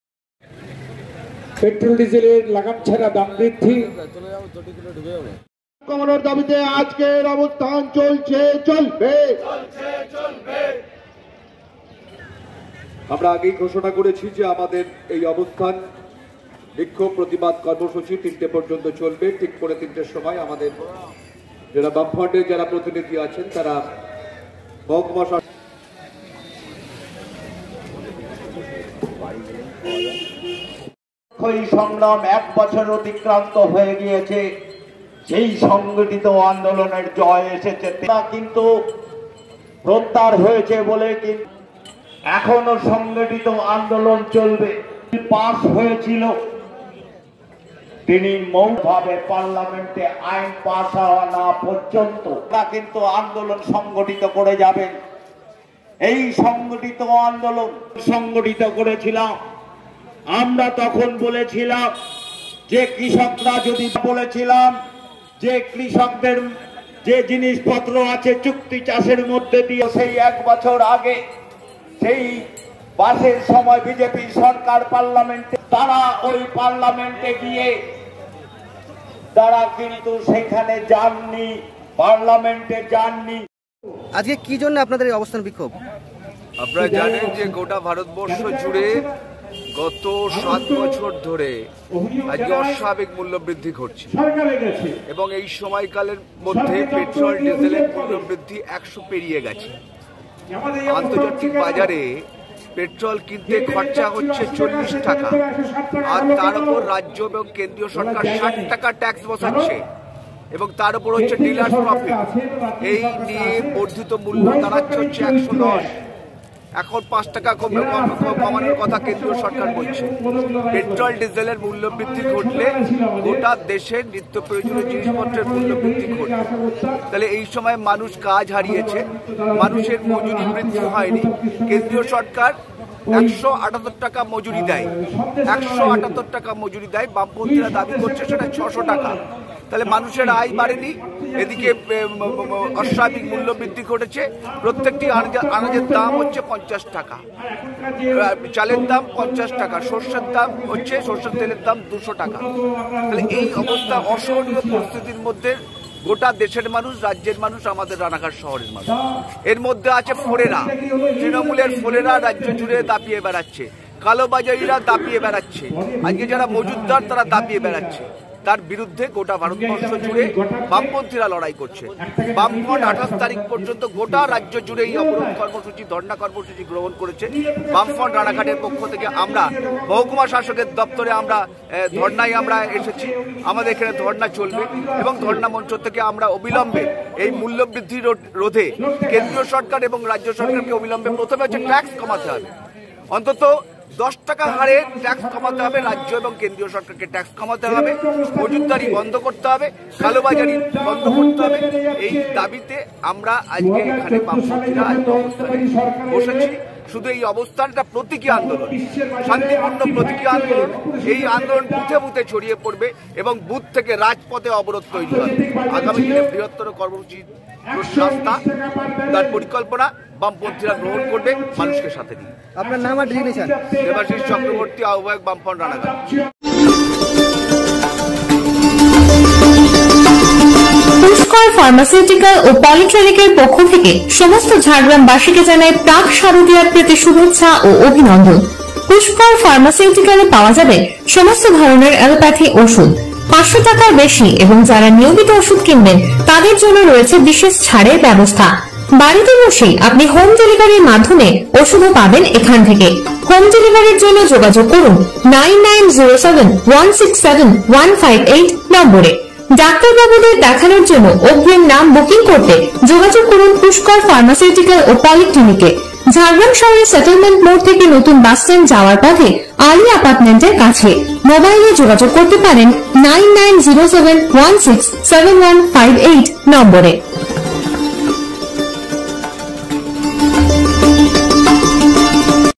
পেট্রোল ডিজেল ও রান্নার গ্যাসের মূল্য বৃদ্ধির প্রতিবাদে মঙ্গলবার রানাঘাটের SDO অফিসের সামনে
পেট্রোল ডিজেল ও রান্নার গ্যাসের মূল্য বৃদ্ধির প্রতিবাদে মঙ্গলবার রানাঘাটের SDO অফিসের সামনে রানাঘাট সিপিএম এর পক্ষ থেকে ডেপুটেশন এবং অবস্থান বিক্ষোভ